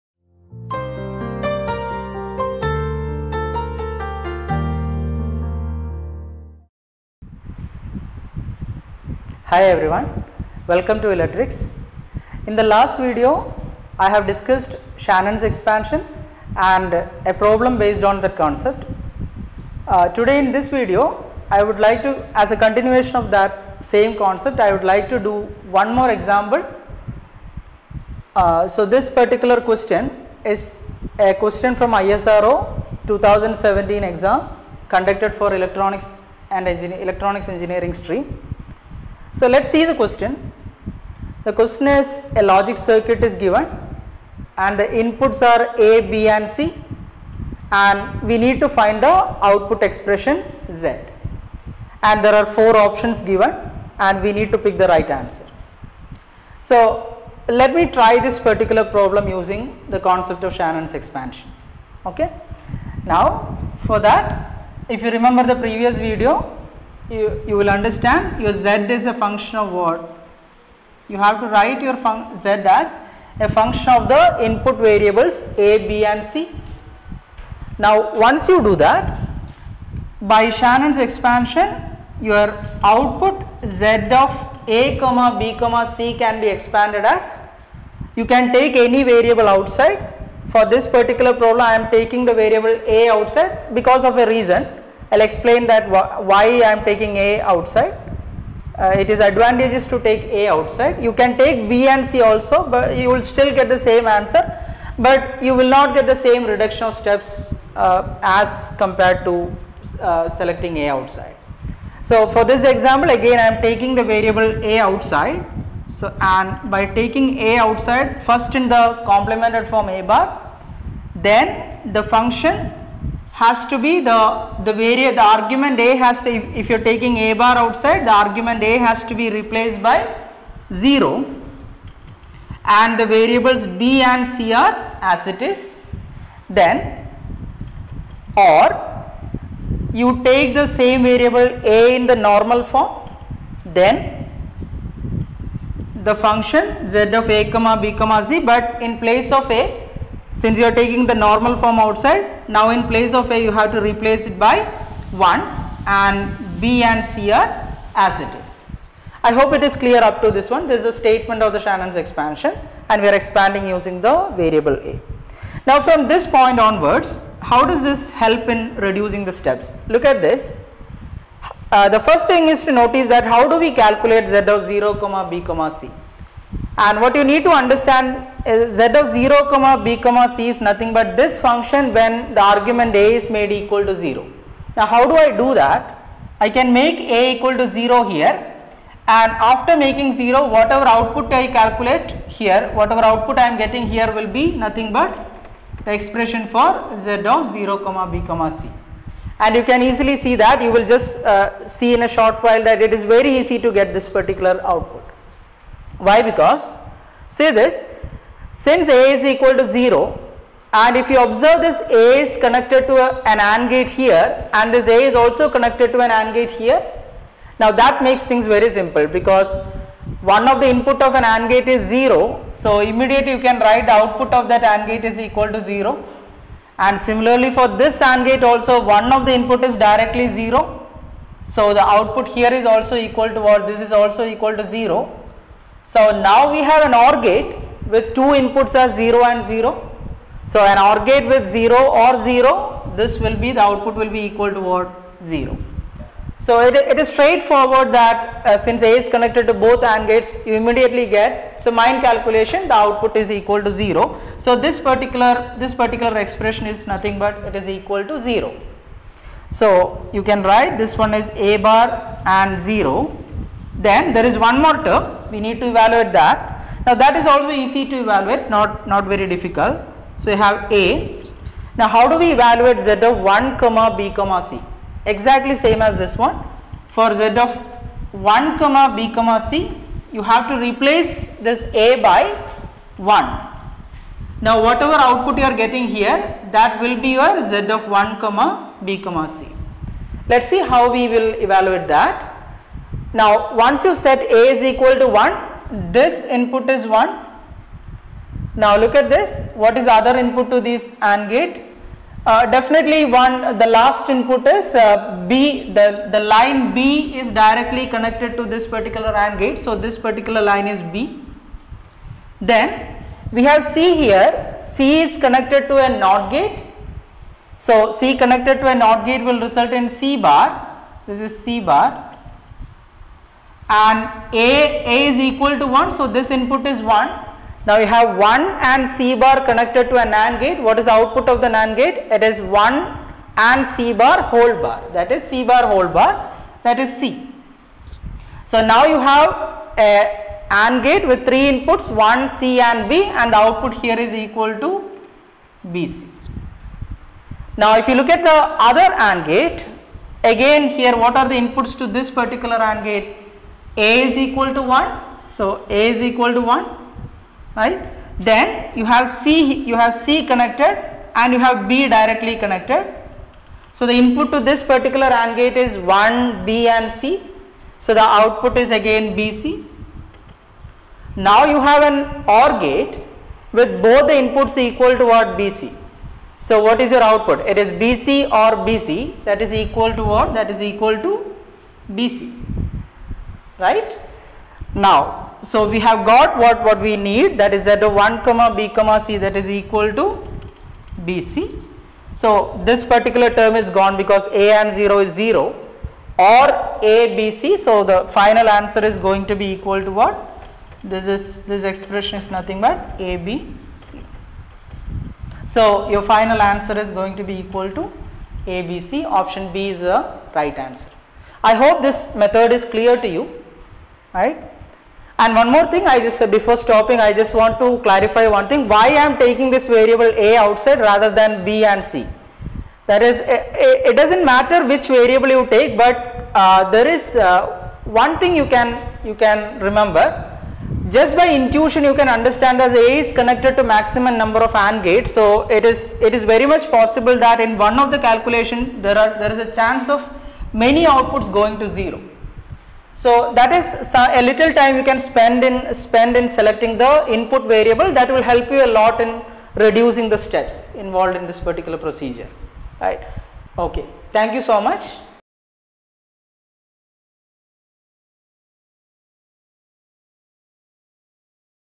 Shannon's Expansion- Example-2
ISRO-2017 Electronics Engineering Question solved using Shannon's Expansion.